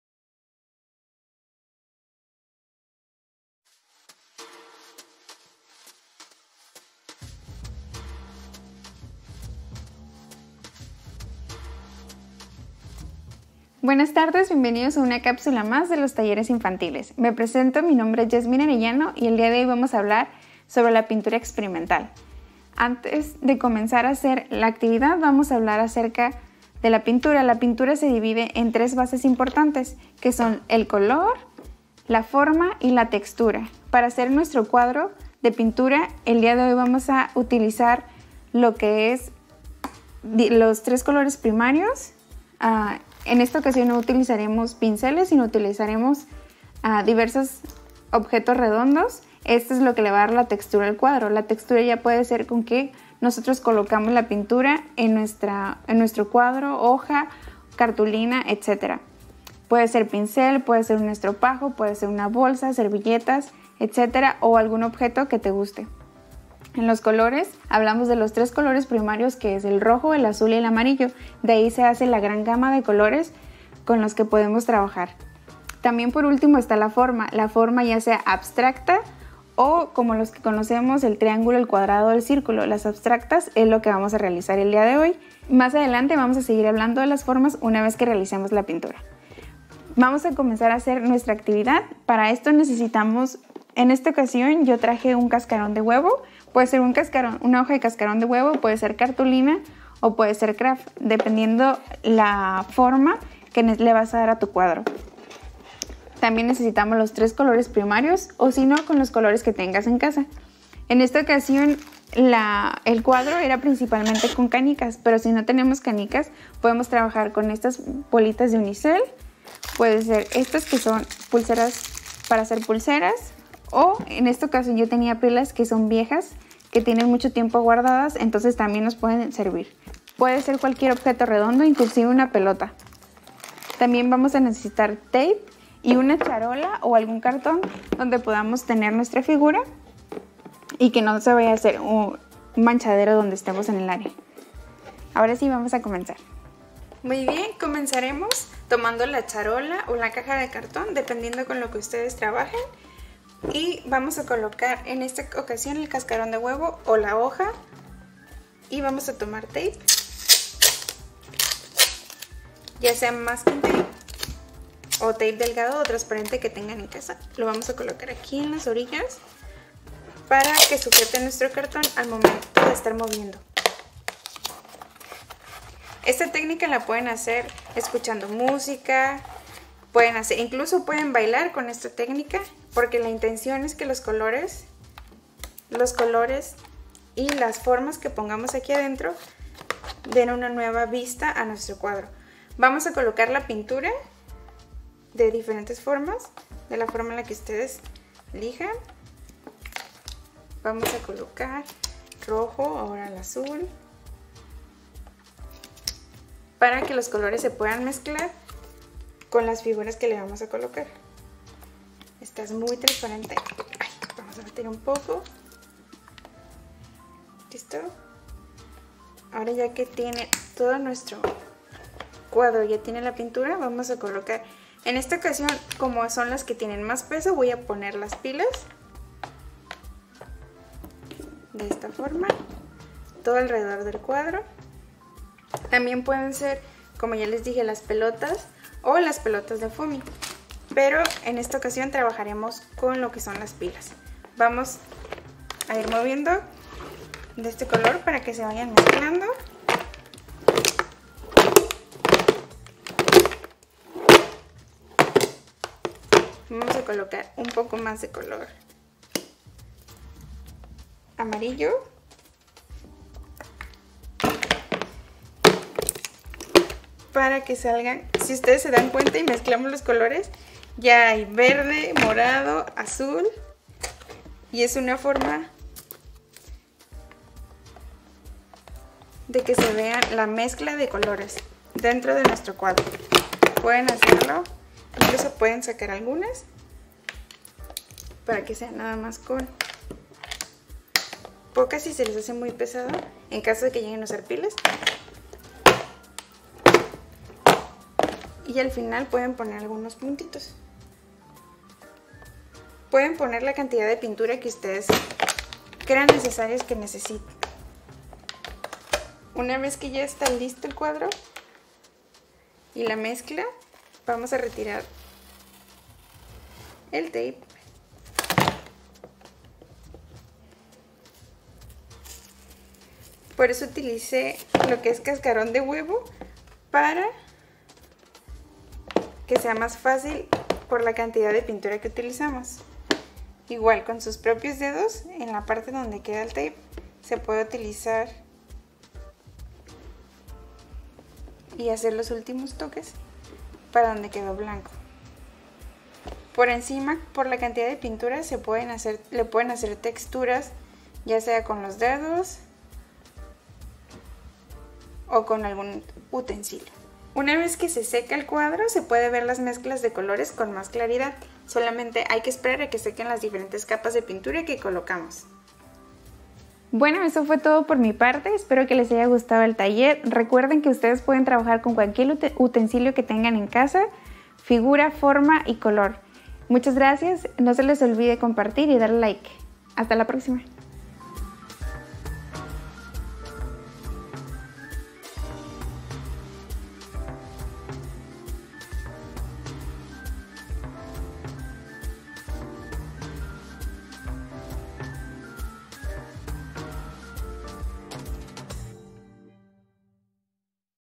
Taller infantil: Pintura experimental con texturas
Hoy lunes de talleres infantiles, Yasmín Arellano nos presenta el taller:
Pintura experimental con texturas.
Comienza un verano divertido, trabajando desde casa con diferentes texturas, colores y formas para crear divertidas obras de arte.